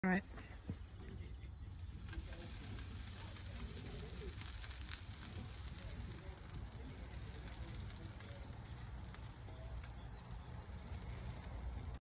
Dan’s Stainless Deep Base oil drum bbq
Dan firing up his stainless deepbase with some lamb on the rotisserie. Creative Oil drum bbqs by Original Jerk from south London have developed a bit of a cult following over the years for their beautifully crafted and highly efficient bbqs